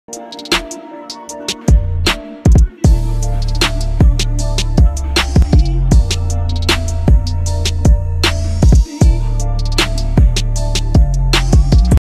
Plug Necklace Fully Iced Out
Fully Iced plug necklace. Available in two different colors, Silver & Gold.
Pendant Size 5.8X2.2CM;
Chain Type: Rope Chain
Chain size 3mm.